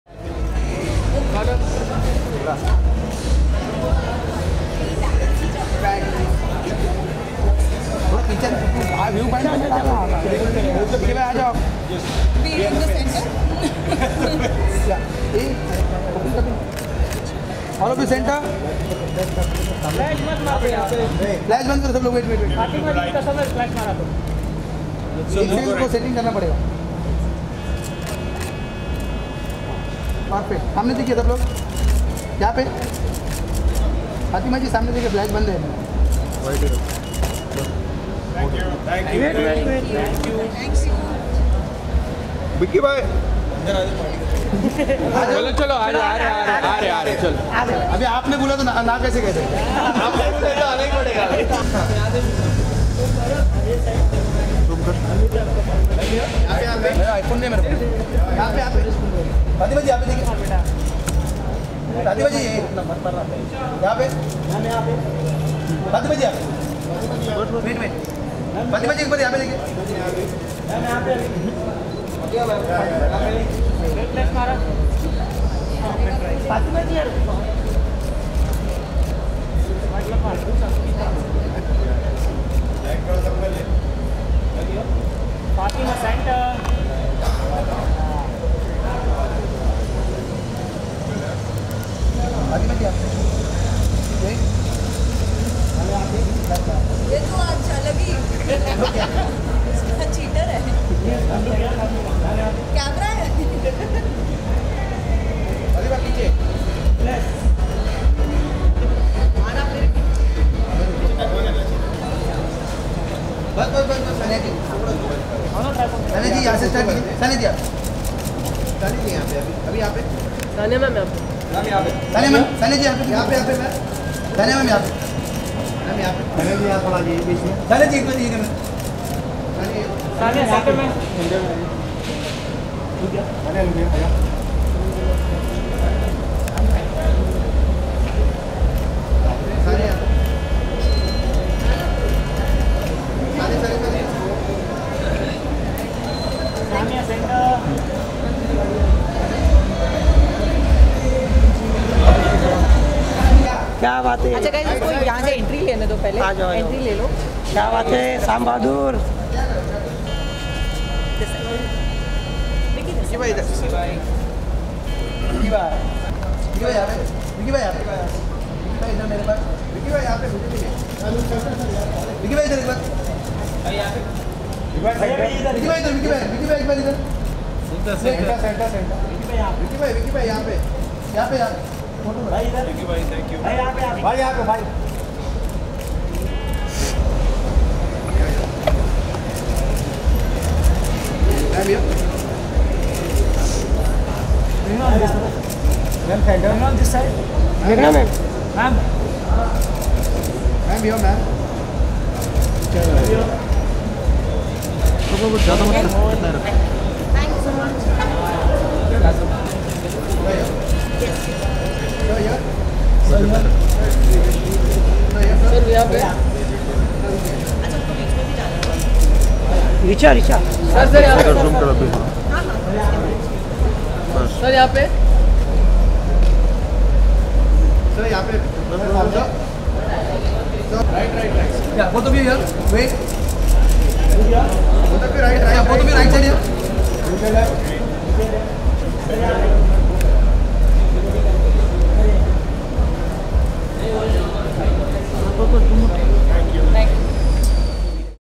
Vicky Kaushal, Fatima And Sanya At The Sam Bahadur Wrap Party
विकी कौशल और बाकी कलाकारों से सजी फिल्म सैम बहादुर की शूटिंग पूरी हो गई है। इस मौके पर एक पार्टी का आयोजन किया गया। जहां फातिमा और सान्या अलग ही लुक में नजर आई।